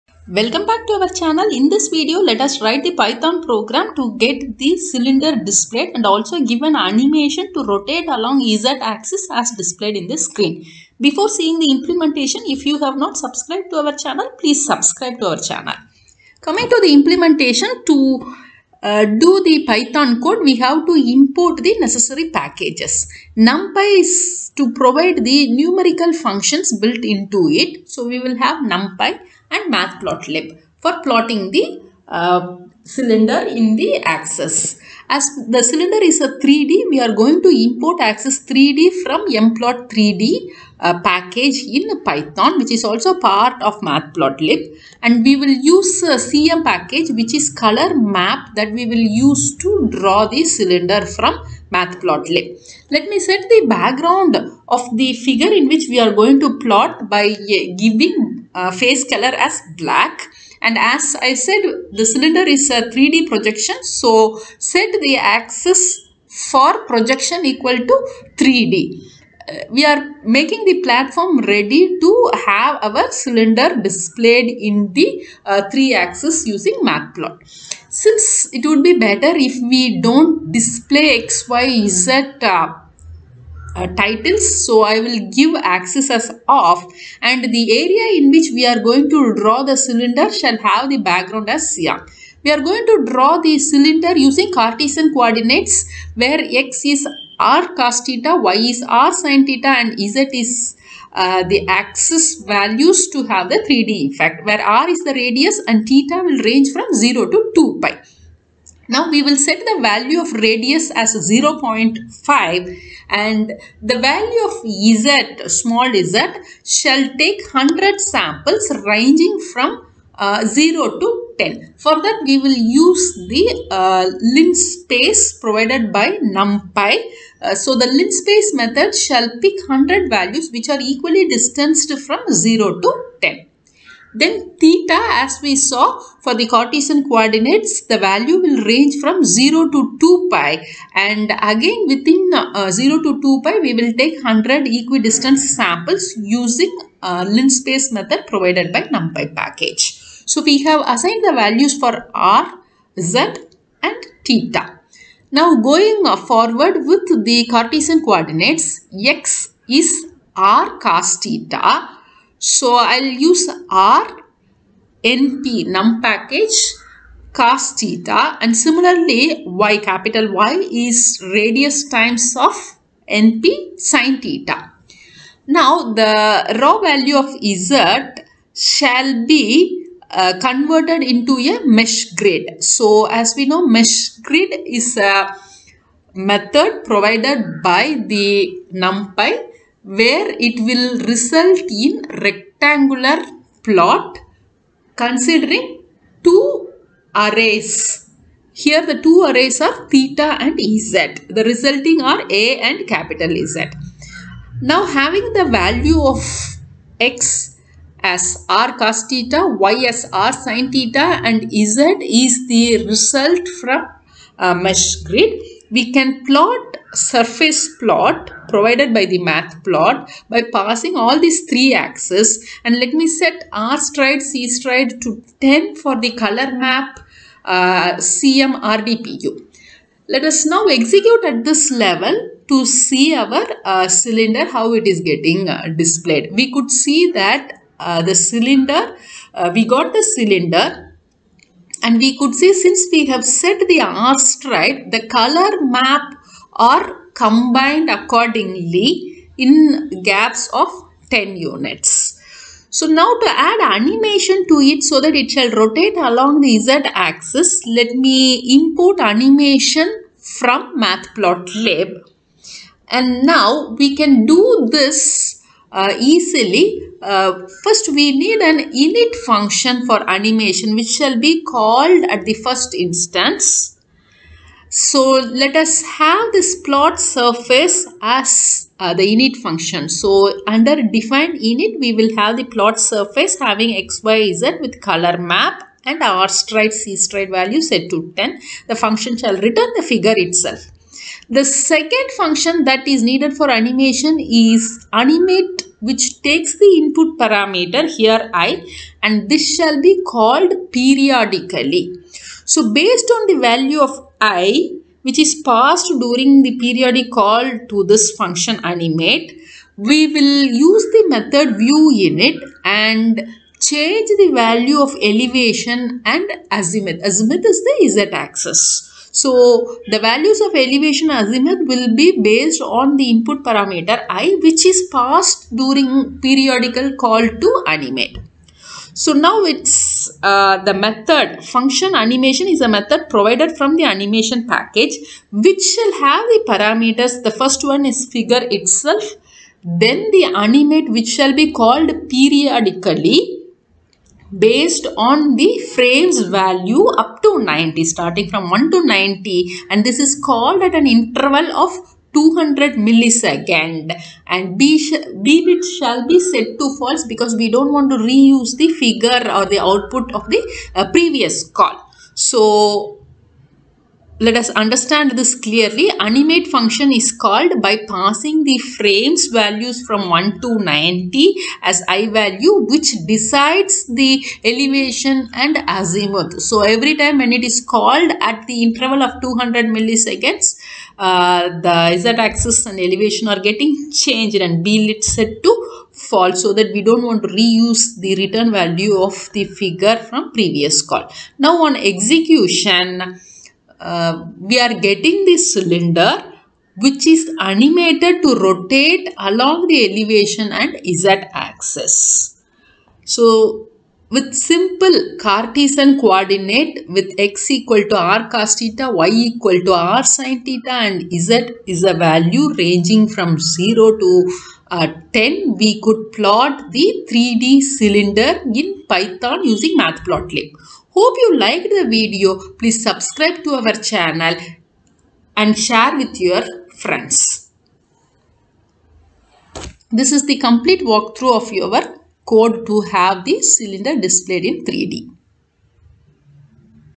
How to draw Cylinder (3D) and rotate with Matplotlib | Python Animation
How to plot Cylinder shape in Python Matplotlib is explained in this video. It also uses FuncAnimation method from animation package to rotate sphere along "Z" axis.